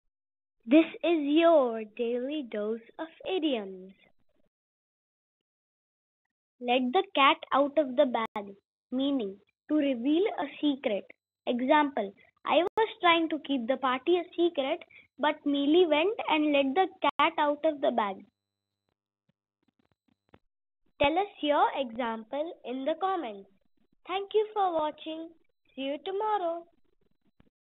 Let the cat out of the bag | Daily Dose of Idioms
Today's idiom is:
Let the cat out of the bag!
Meaning:
To reveal a secret
Example:
I was trying to keep the party a secret, but Mili went and let the cat out of the bag.